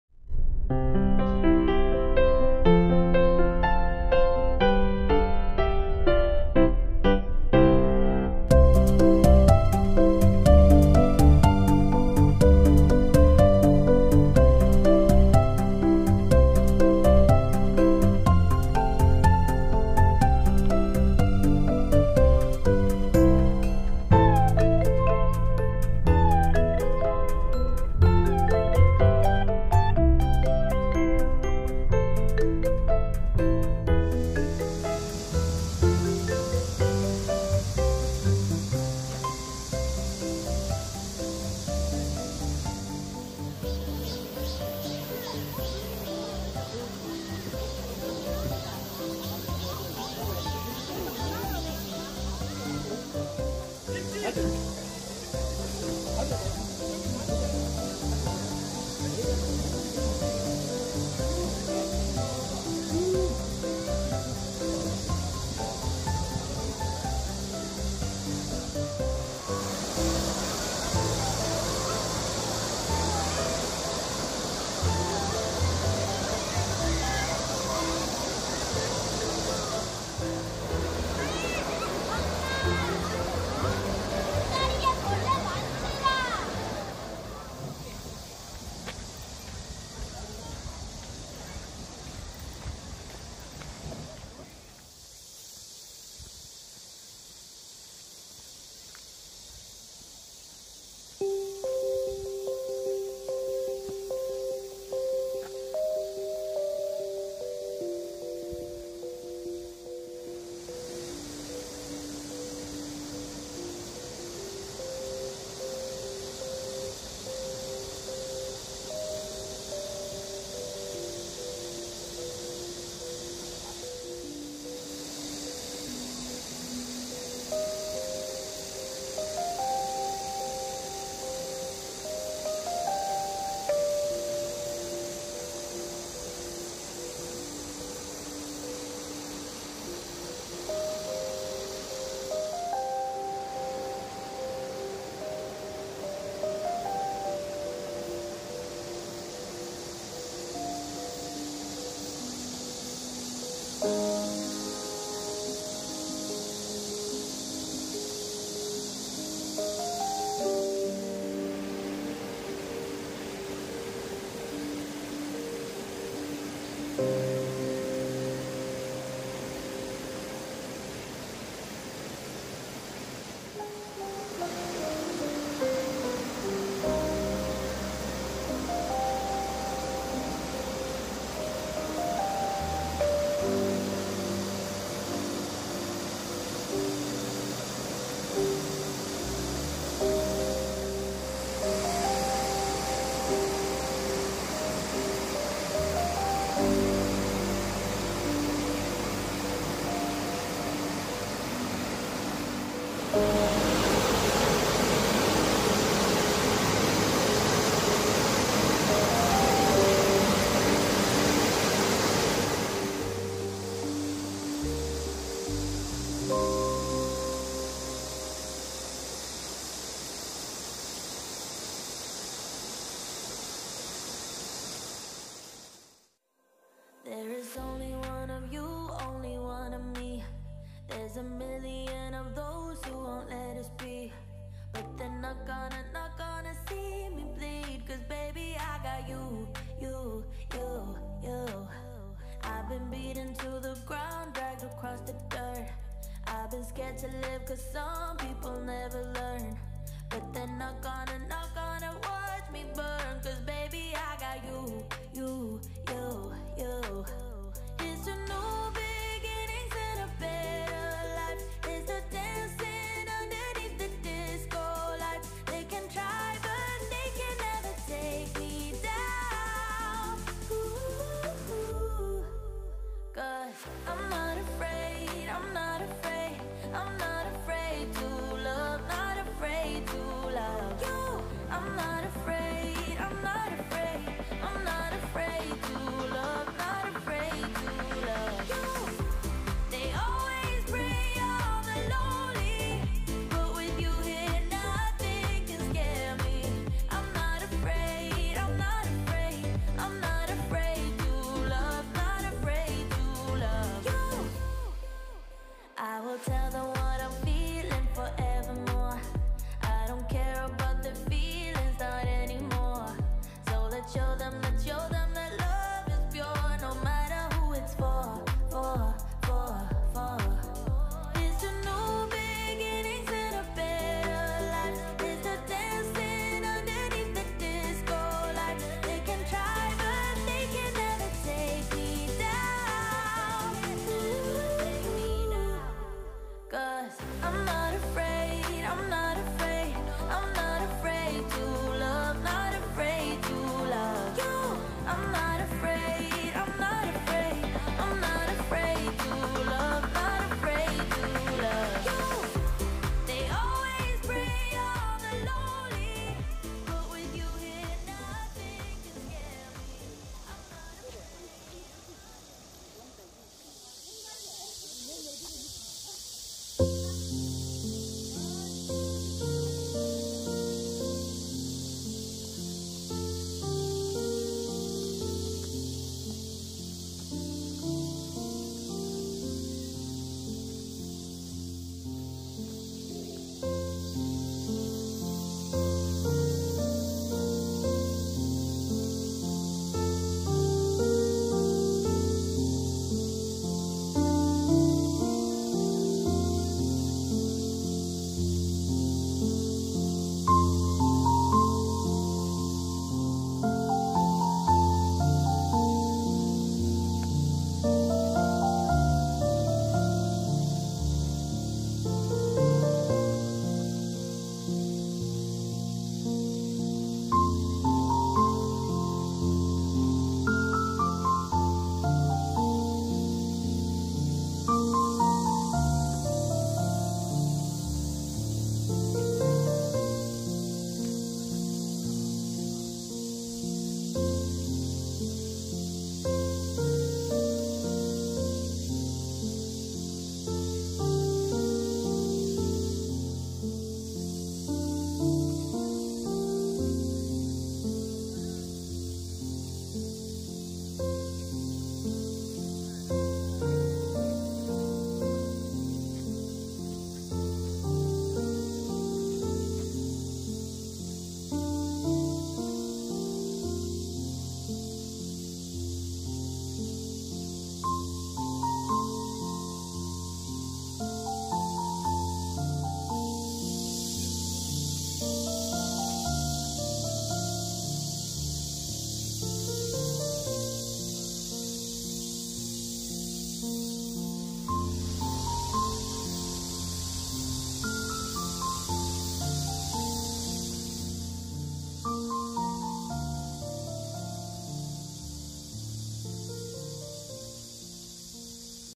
[경남 거창]수승대 출렁다리 올여름 피서 딱입니다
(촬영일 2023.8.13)
경남 거창군 위천면 황산리 890번지
빼어난 경치로 선비들의 마음을 단 번에 사로잡는 곳이 있었으니 바로 거창의 수승대다

거창 수승대 출렁다리
작년에 완공된 출렁다리로
하절기( 3월 ~ 10월 )
10 : 00 ~ 17 : 50
동절기( 11월 ~ 2월 )
10 : 00 ~16 : 50
입장료, 주차료 무료
매주 월요일은 정기휴무에요
길이 240m
폭 1.5m